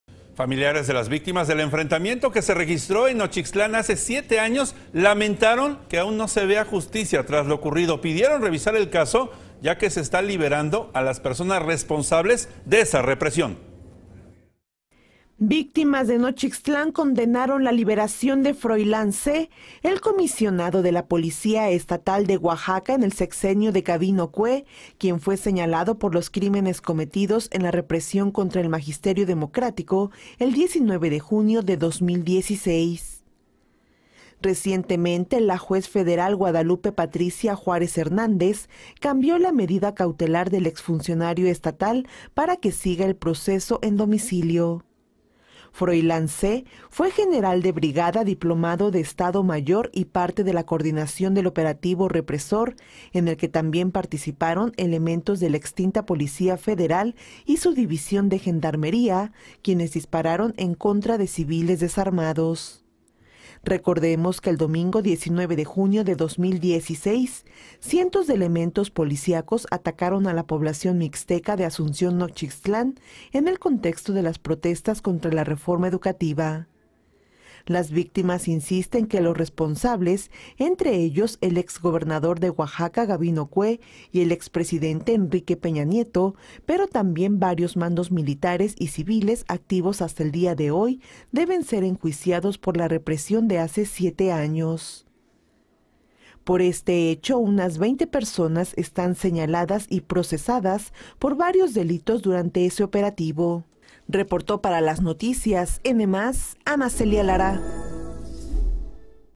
Piden Justicia para Víctimas de Nochixtlán | Las Noticias Oaxaca -🔴 Noticias en vivo
Denuncia | Familiares de las víctimas del enfrentamiento que se registró en Nochixtlán hace 7 años lamentaron que aún no se vea justicia tras lo ocurrido, pidieron revisar el caso ya que se ha dado la liberación de personas responsables de la represión.

Con información de: 🎙️ Ana Celia Lara